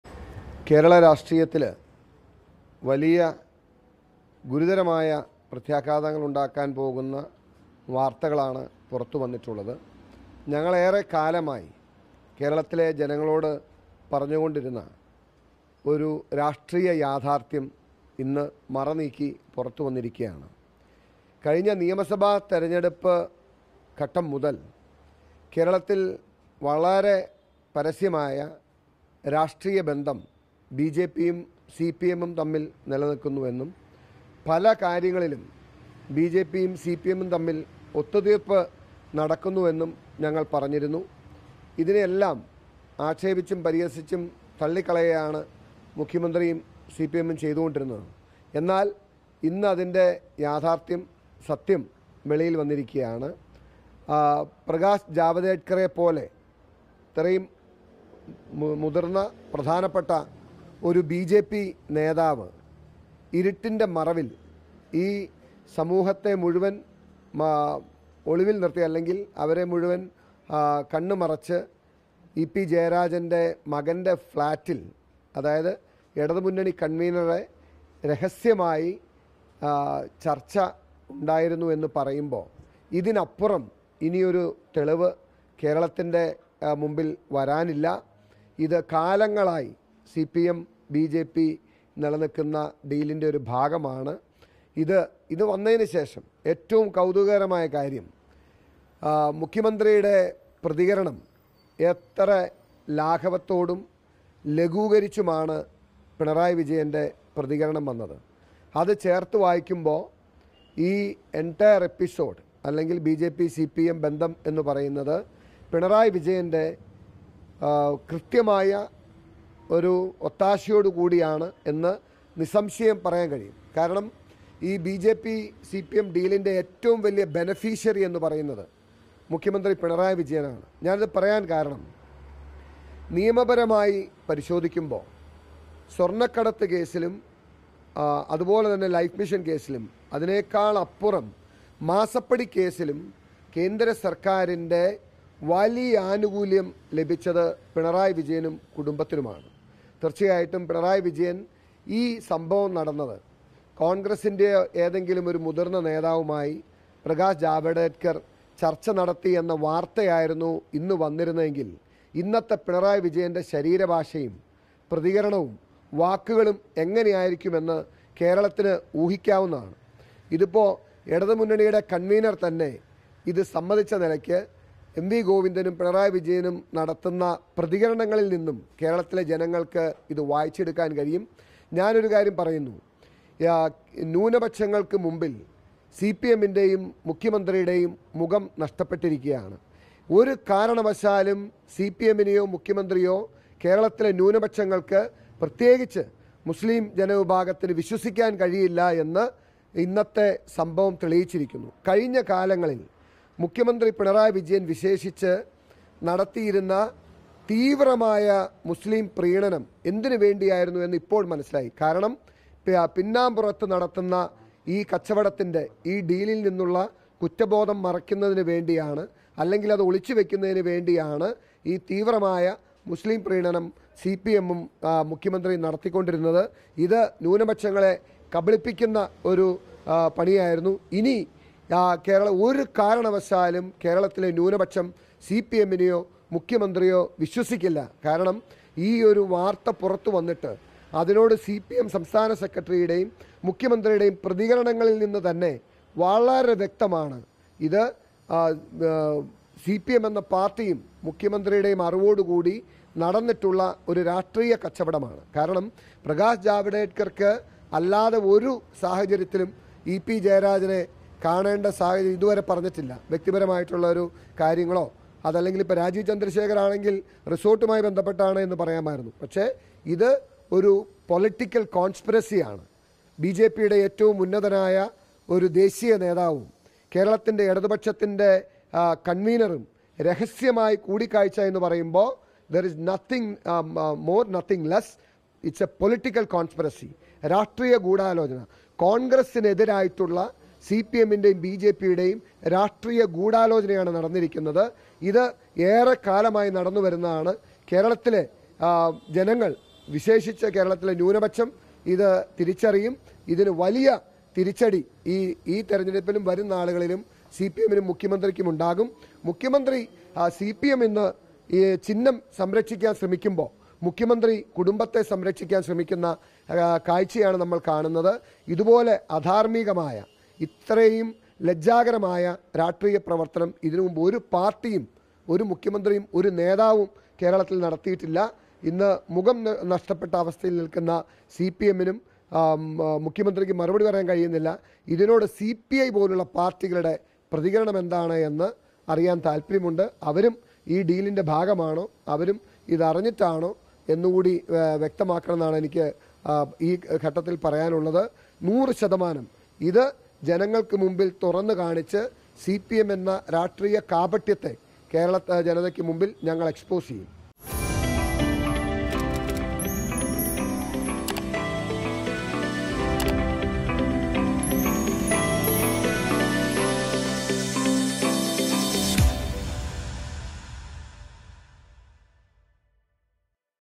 'ബിജെപിയുമായി ചർച്ച നടത്തി സിപിഎം ന്യൂനപക്ഷങ്ങളെ വഞ്ചിച്ചു'
'സിപിഎം ബിജെപി ഡീലിൽ ഏറ്റവും വലിയ ആനുകൂല്യം കിട്ടിയത് പിണറായി വിജയന്, ന്യൂനപക്ഷങ്ങൾക്ക് മുന്നിൽ സിപിഎമ്മിന്‍റെയും പിണറായിയുടെയും മുഖം നഷ്ടപ്പെട്ടു, മുസ്ലീം വിഭാഗത്തിന് സിപിഎമ്മിനെ വിശ്വസിക്കാനാവില്ല'